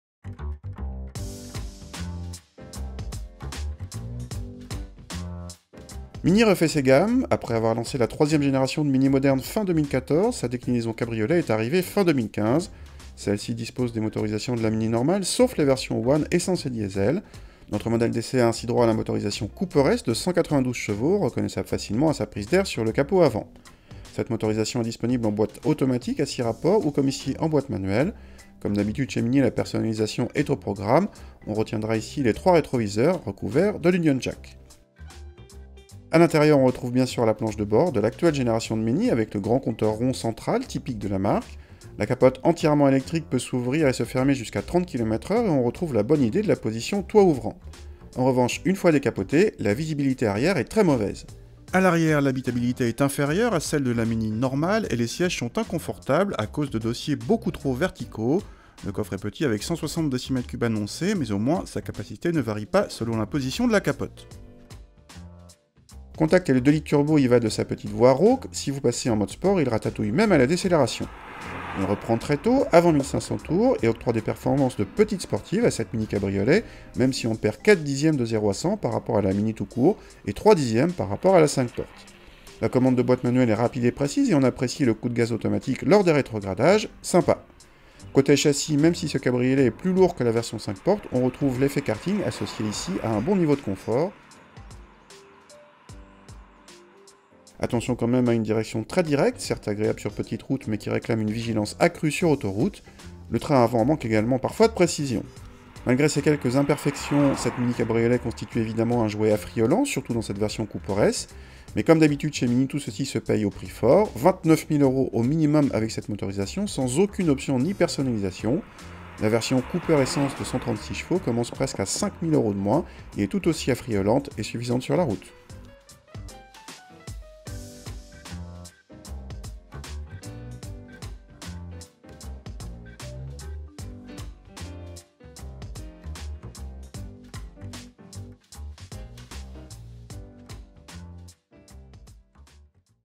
Mini Cabrio Cooper S 2016
Mini refait ses gammes : après avoir lancé la troisième génération de Mini moderne fin 2014, sa déclinaison cabriolet est arrivée fin 2015. Celle-ci dispose des motorisations de la Mini normale, sauf les versions One essence et diesel. Notre modèle d’essai a ainsi droit à la motorisation Cooper S de 192 ch, reconnaissable facilement à sa prise d’air sur le capot avant. Cette motorisation est disponible en boîte automatique à 6 rapports ou comme ici en boîte manuelle. Comme d’habitude chez Mini, la personnalisation est au programme : on retiendra  ici les 3 rétroviseurs recouverts de l’Union Jack…

A l’intérieur, on retrouve bien-sûr la planche de bord de l’actuelle génération de Mini, avec le grand compteur rond central typique de la marque. La capote entièrement électrique peut s’ouvrir et se fermer jusqu’à 30 km/h et on retrouve la bonne idée de la position toit ouvrant. En revanche, une fois décapoté, la visibilité arrière est mauvaise. A l’arrière, l’habitabilité est inférieure à celle de la Mini normale et les sièges sont inconfortables à cause de dossier beaucoup trop verticaux. Le coffre est petit avec 160 dm3 annoncés, mais au moins sa capacité ne varie pas selon la position de la capote.

Contact et le 2 litres turbo y va de sa petite voix rauque : si vous passez en mode sport, il ratatouille même à la décélération ! Il reprend très tôt, avant 1500 tours, et octroie des performances de petite sportive à cette Mini cabriolet même si on perd 4 dixièmes au 0 à 100 par rapport à la Mini tout court, et 3 dixièmes par rapport à la 5 portes. La commande boîte manuelle est rapide et précise et on apprécie le coup de gaz automatique lors des rétrogradages : sympa ! Côté châssis, même si ce cabriolet est plus lourd que la version 5 portes, on retrouve l’effet karting associé ici à un bon niveau de confort.

Attention quand même à une direction très directe, certes agréable sur petite route, mais qui réclame une vigilance accrue sur autoroute. Le train avant manque également parfois de précision. Malgré ces quelques imperfections, cette Mini cabriolet constitue évidemment un jouet affriolant, surtout dans cette version Cooper S. Mais, comme d’habitude chez Mini, tout ceci se paye au prix fort : 29000 € au minimum avec cette motorisation sans aucune option ni personnalisation. La version Cooper essence de 136 ch commence à presque 5000 € de moins, et est tout aussi affriolante et suffisante sur la route.